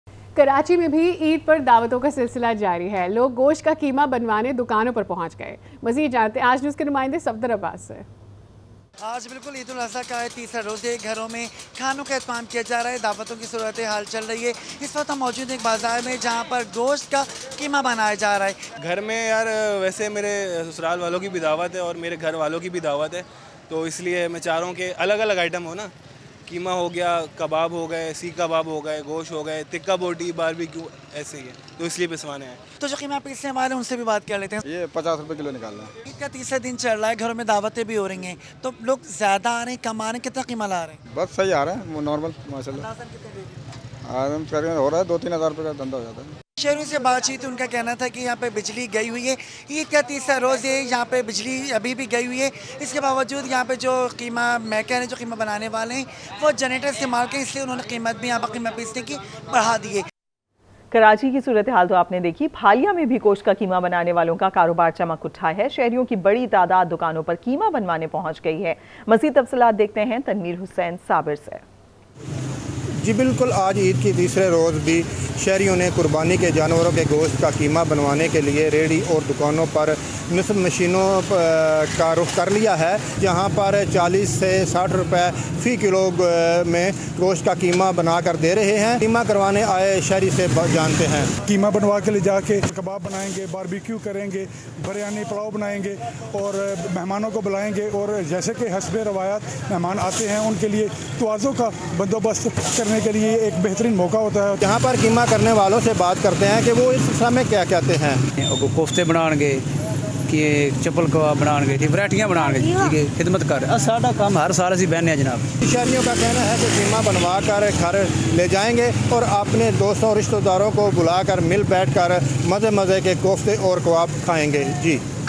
Eid ul Adha 3rd day - series of feasts continue - people reached meat shops to make Qeema - Aaj News
Eid ul Adha 3rd day - series of feasts continue - people reached meat shops to make Qeema - 1 July 2023- Aaj News

Spot Light With Munizae Jahangir:
shorturl.at/gwFU6